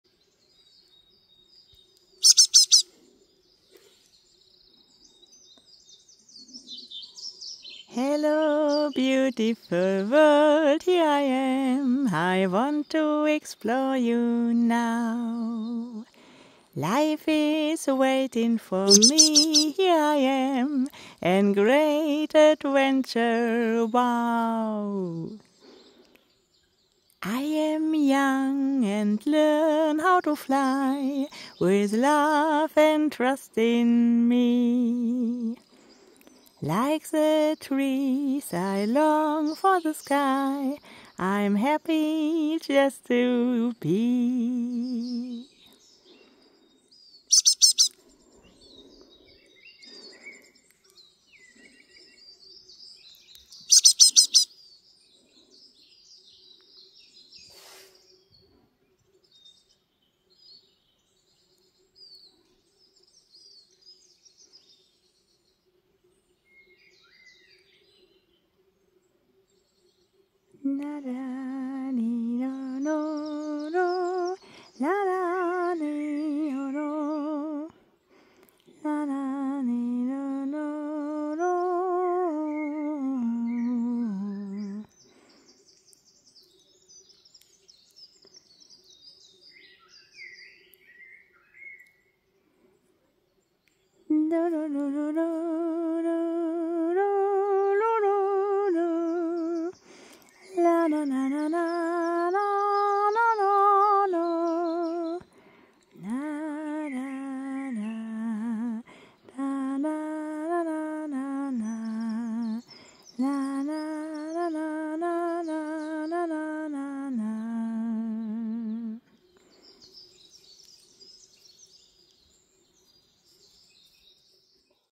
Singing for this little Cutie / Gesang für diese junge süße Kohlmeise
Isn't it a lttle cutie my dear friends? This young great tit hopped around and was sitting for a while in the grass, where I sang and talked to it. I felt a wonderful connection between us. Yes, this world is an great adventure for all the young birdies.

Ist das nicht eine süße Kleine meine lieben Freunde? Die junge Kohlmeise hüpfte durch das Gras und saß dann eine Weile dort. Ich sprach mit ihr, sang für sie und fühlte eine wundervolle Verbindung zwischen uns. Ja, diese Welt ist ein großes Abenteuer für all die jungen Vögelchen.